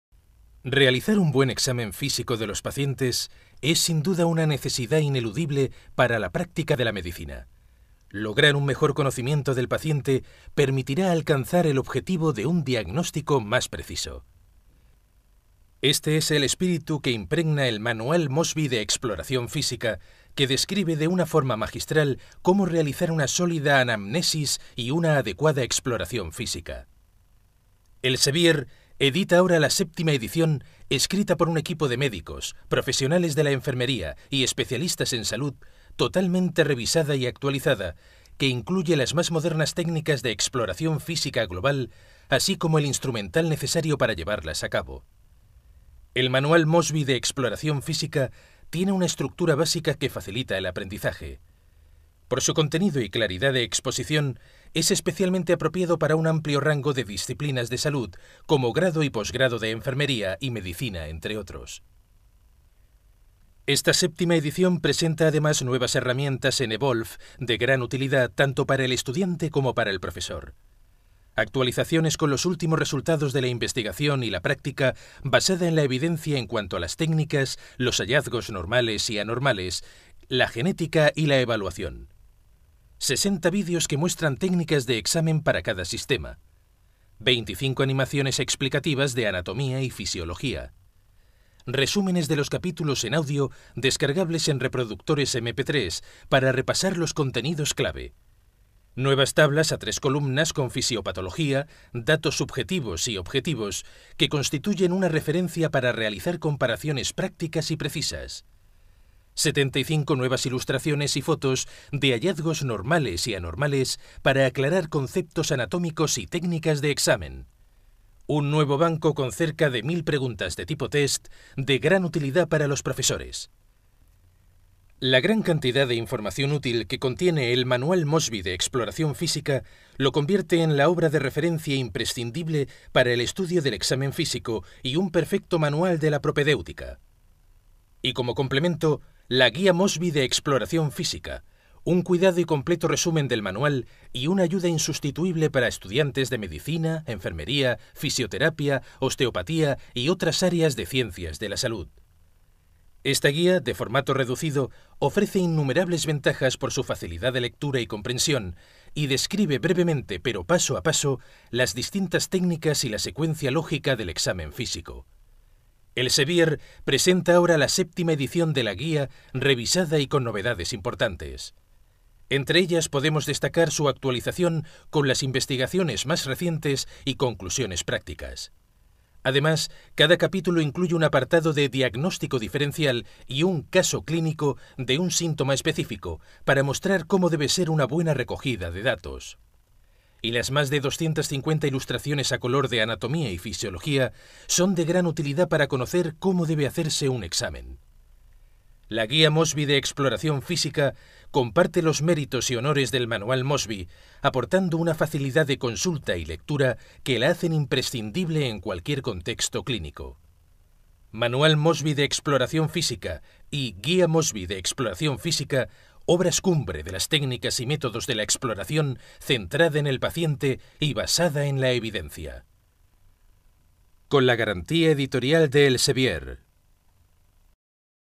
Exploración Física: Manual y Guía Mosby (Seidel)
El Manual Mosby de Exploración Física ofrece una visión completa de las técnicas y métodos de la exploración centrada en el paciente y basada en la evidencia.  El contenido se estructura en base a los siguientes parámetros; anatomía y fisiología, revisión de la historia clínica, examen y hallazgos y anormalidades, y ofrece más de 1.200 ilustraciones, además de numerosas tablas y cuadros.

La Guía Mosby de Exploración Física comparte los méritos y honores del Manual Mosby, aportando una facilidad de consulta y lectura que la hacen imprescindible en cualquier contexto clínico. Práctico resumen, en un formato de fácil manejo y consulta que incluye breves descripciones de las técnicas de exploración y directrices, expuestas paso a paso.

¡Obras fundamentales en el estudio de la Exploración Física!